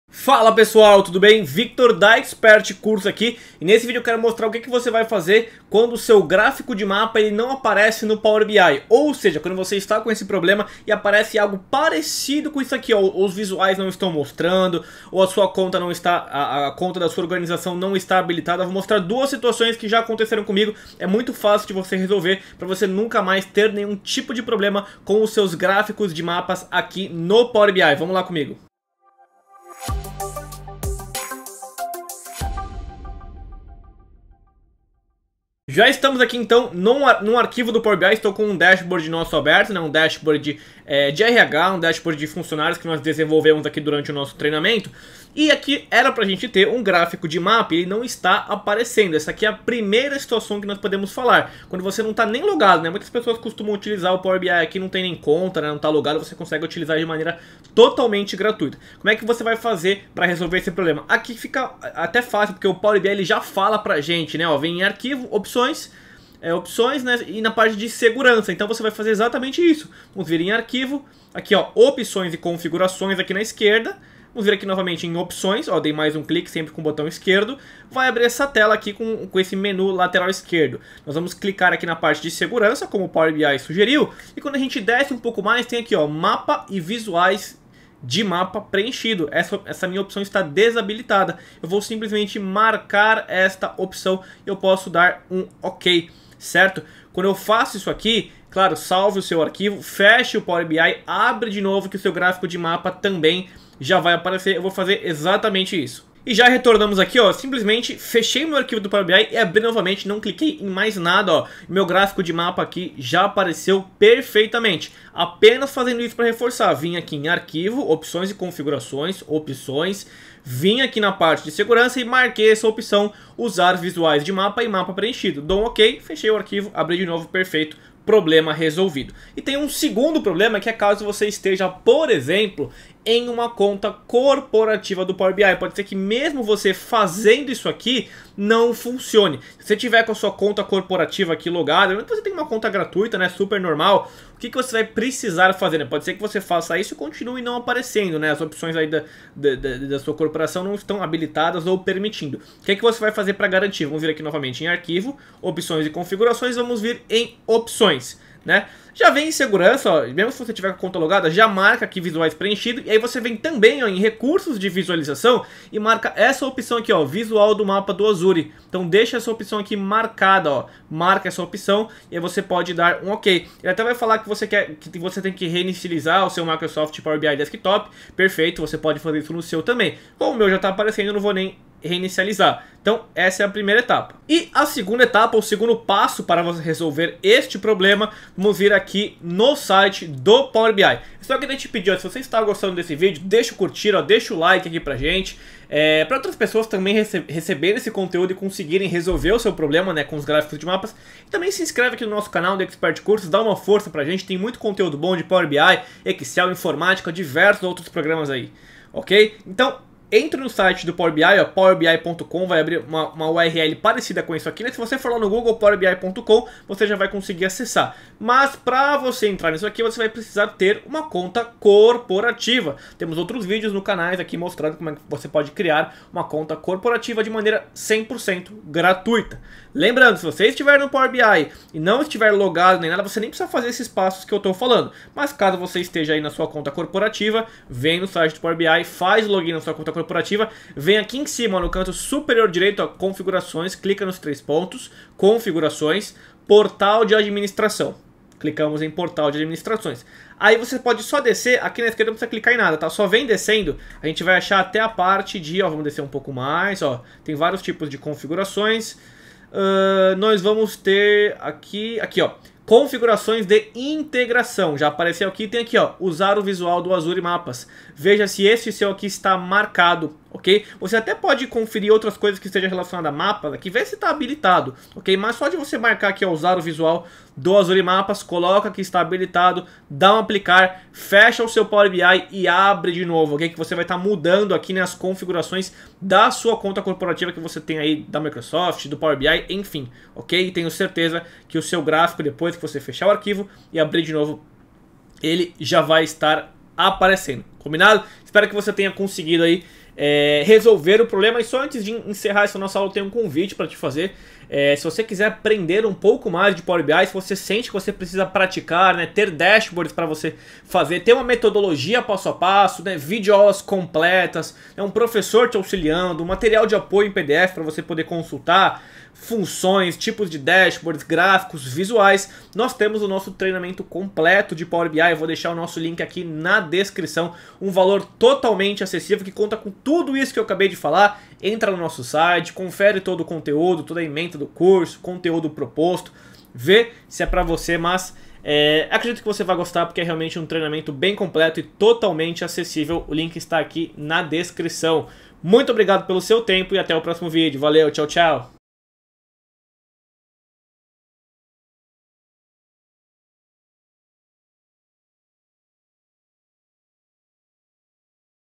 Gráfico de Mapa NÃO aparece no Power BI (Resolvido)
Entenda como solucionar o problema quando o seu gráfico de mapa não aparece na visualização do Power BI

É comum a visualização do gráfico de mapa não aparece no Power BI, e basicamente existem duas coisas que podem ser feitas para corrigir esse "erro" visual.

Tudo vai depender se você está ou não logado em sua conta do Power BI, nesse vídeo mostramos como corrigir tudo isso.

O gráfico de mapa é um visual muito interessante, deixa o Dashboard muito mais vivo e bonito.

► Ative as notificações e curta o nosso vídeo